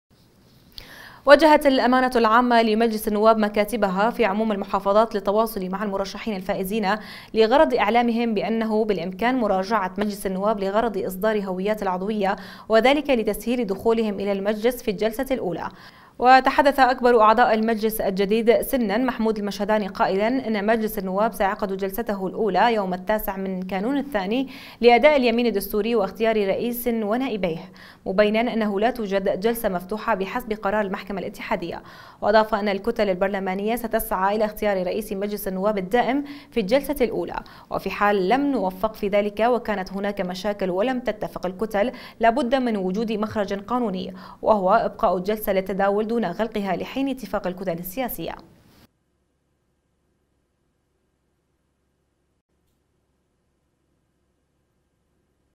البرلمان يمنح أعضائه الهويات لحضور الجلسة الأولى المفتوحة أو المستمرة
وجهت الأمانة العامة لمجلس النواب مكاتبها في عموم المحافظات للتواصل مع المرشحين الفائزين لغرض إعلامهم بأنه بالإمكان مراجعة مجلس النواب لغرض إصدار هويات العضوية وذلك لتسهيل دخولهم الى المجلس في الجلسة الاولى،
و تحدث أكبر أعضاء المجلس الجديد سناً محمود المشهداني قاءلا: إن مجلس النواب سيعقد جلسته الاولى يوم التاسع من كانون الثاني لاداء اليمين الدستوري واختيار رئيس ونائبيه ، مبيناً أنه لا توجد جلسة مفتوحة بحسب قرار المحكمة الاتحادية، وأضاف، أن "الكتل البرلمانية ستسعى إلى اختيار رئيس مجلس النواب الدائم في الجلسة الاولى، وفي حال لم نوفق في ذلك وكانت هناك مشاكل ولم تتفق الكتل لا بد من وجود مخرج قانوني، وهو إبقاء الجلسة للتداول دون غلقها لحين اتفاق الكتل السياسية.